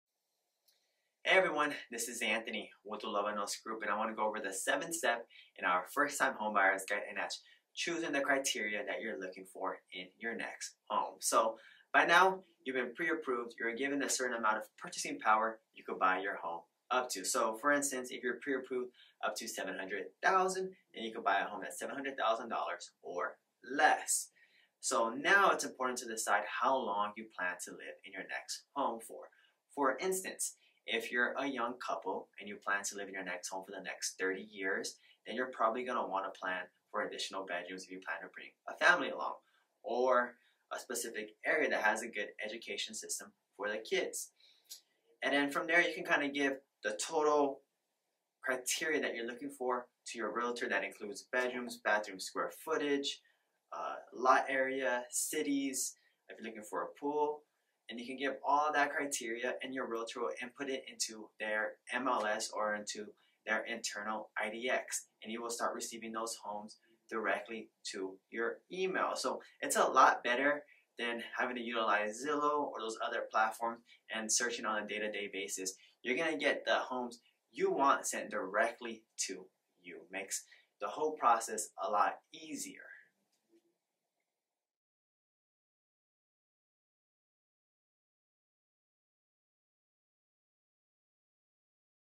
Choose the criteria you are looking for in your next home
When choosing the criteria you are looking for in your next home, do not forget to evaluate how long you plan to live there. This will alter your criteria whether you are a young couple planning to build a family or an older couple preparing to downsize.